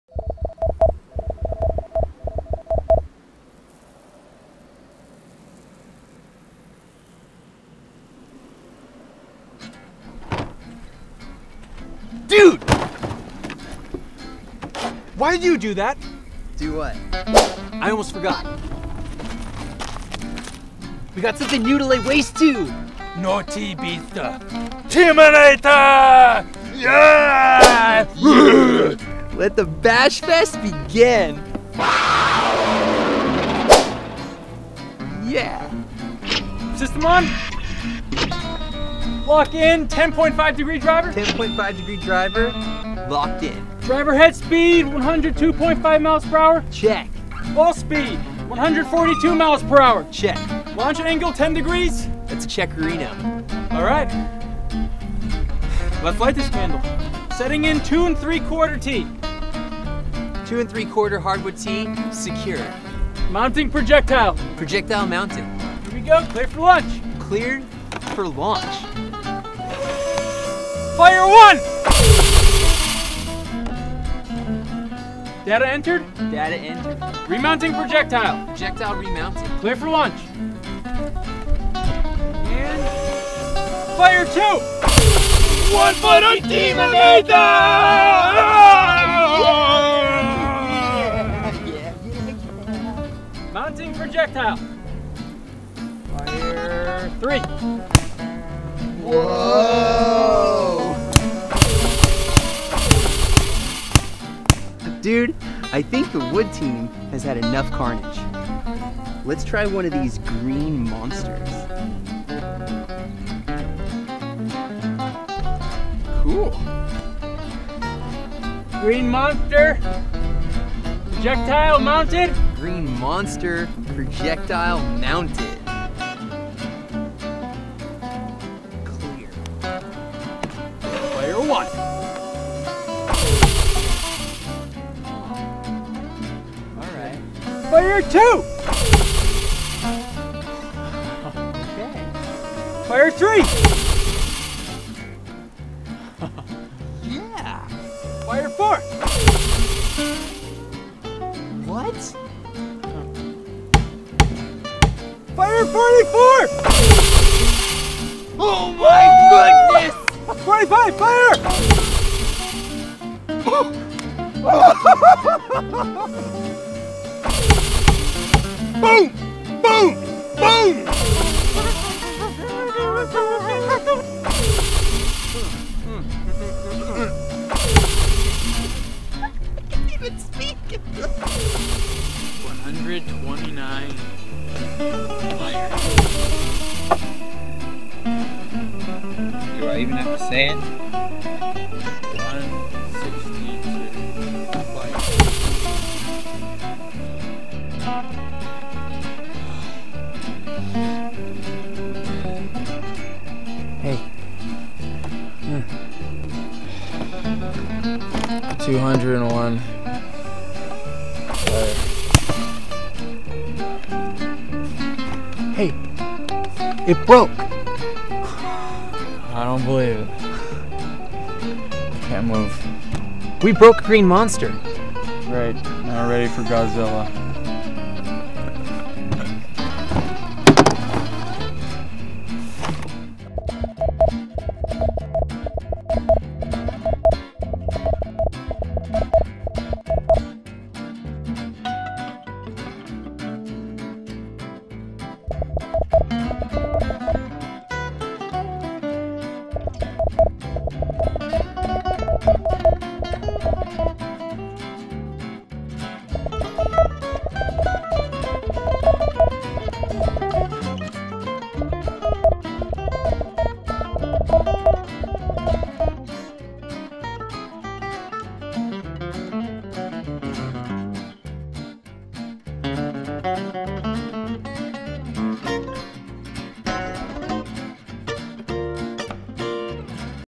Ungodly Strong Green Monsters Bamboo Tees
A fun look at Green Monsters testing results.
We put Green Monsters tees to the test and proved them seven times stronger than leading hardwood tees.